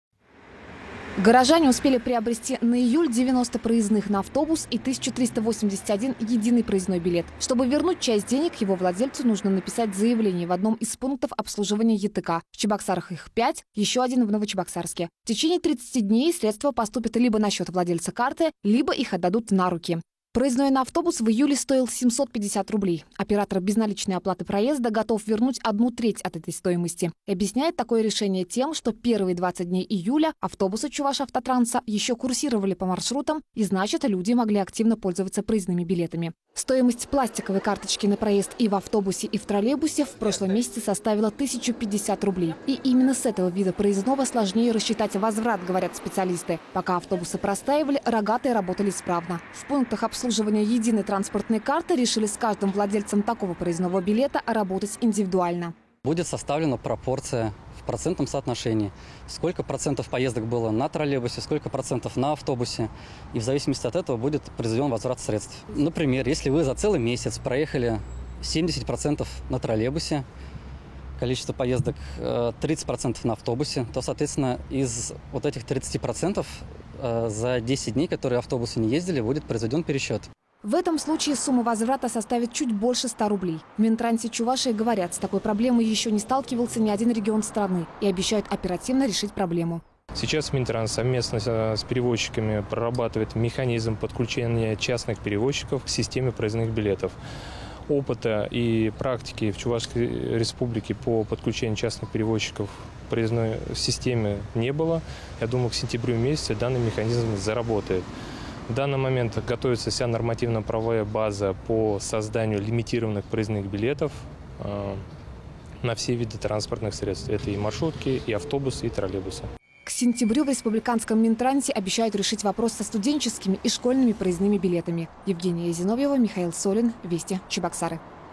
Часть денег за купленные проездные на автобусы «Чувашавтотранса» можно вернуть
Деньги за купленные проездные на автобусы «Чувашавтотранса» можно вернуть. Но только часть за неиспользованные поездки.